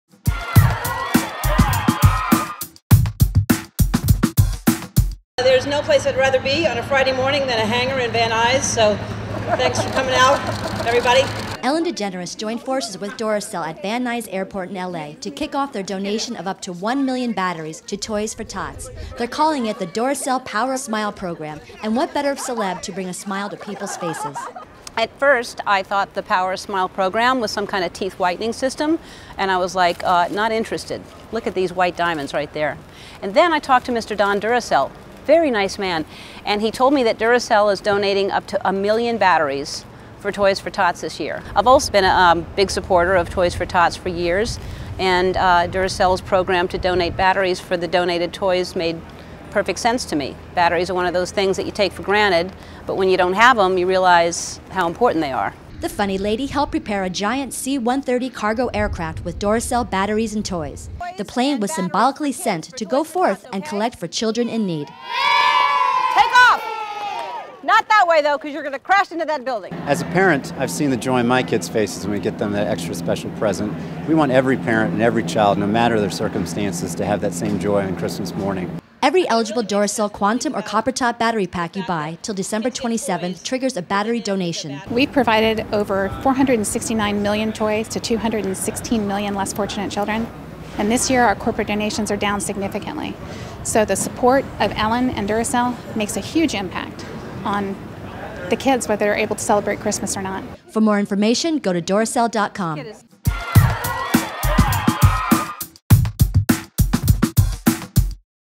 Ellen DeGeneres with Duracell Donates 1 Million Batteries to Toys for Tots
Ellen DeGeneres joined forces with Duracell to kick off their donation of up to 1 million batteries to Toys for Tots at Van Nuys Airport in Los Angeles today.